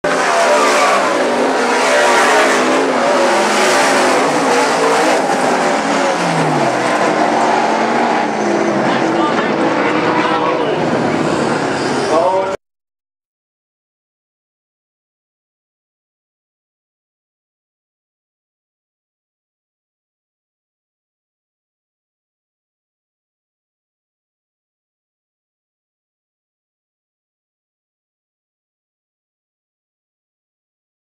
9-25-09 Jason Bloodgood & Travis Jacobson Flip Central Washington State Fair Raceway
9-25-09 Jason Bloodgood & Travis Jacobson Flip Central Washington State Fair Raceway